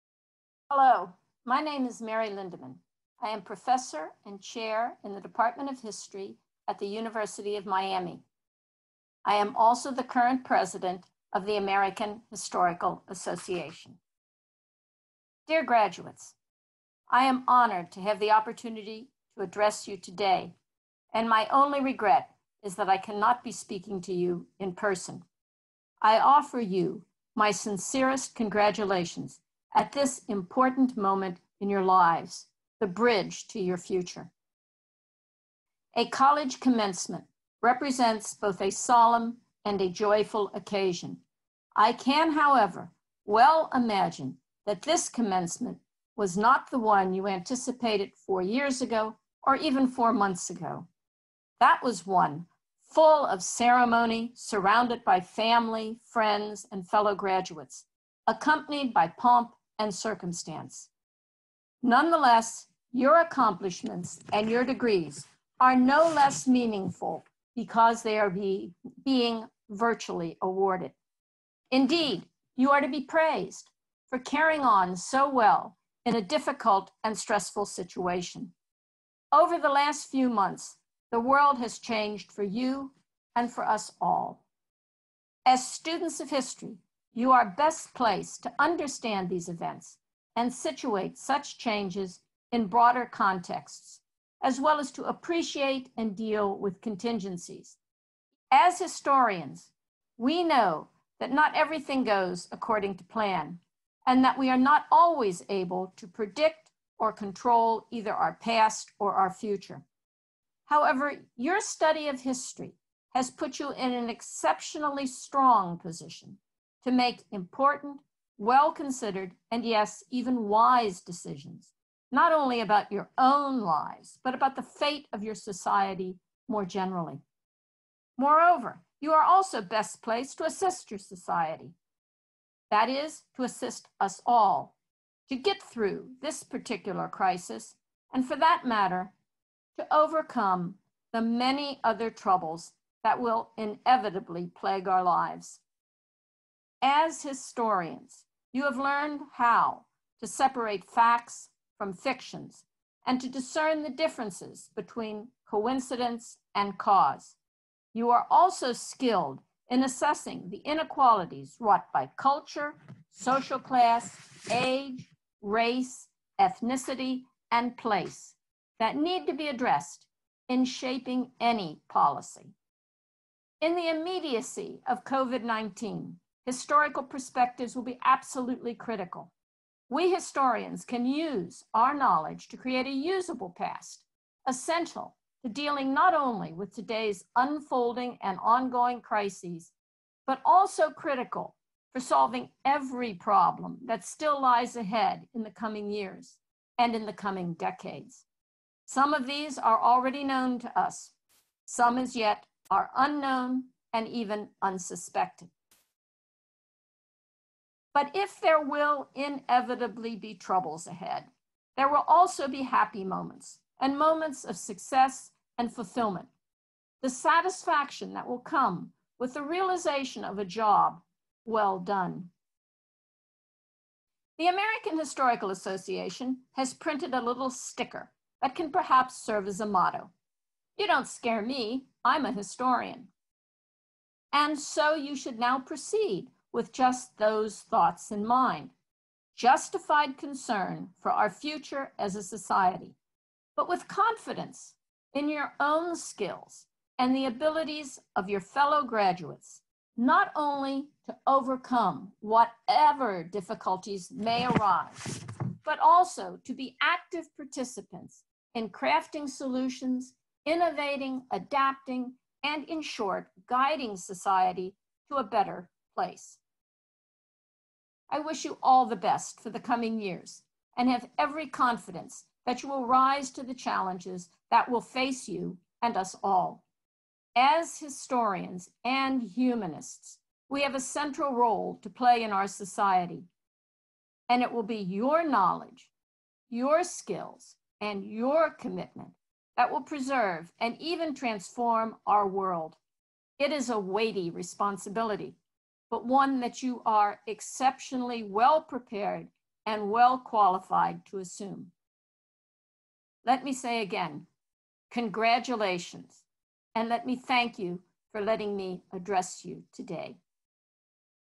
Mary Lindemann's Graduation Speech to the Class of 2020
Dr. Mary Lindemann is professor of history and chair of the Department of History at the University of Miami, where she has taught since 2004 and where she offers courses on the history of early modern European, urban, and medical history. She is currently serving as the President of the American Historical Association. In this video, she speaks to the history majors graduating in spring 2020.